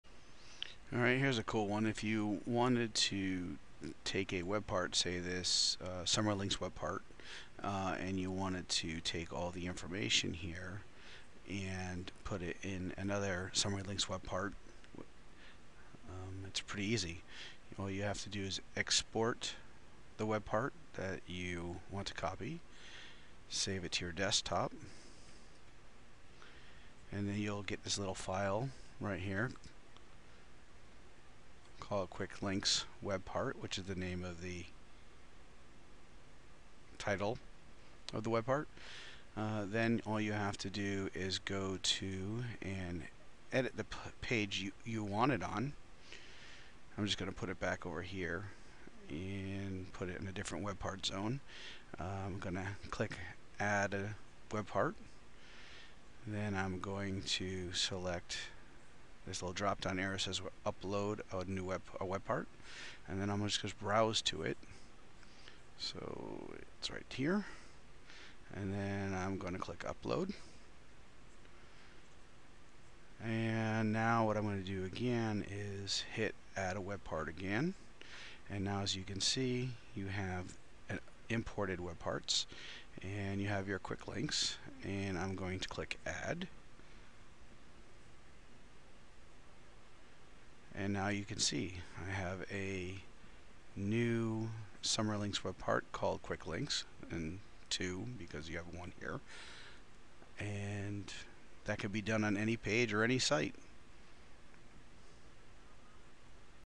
SharePoint - Export and import a web part
Use this method if you need a duplicate of the same web part and its contents on another page or sub-site.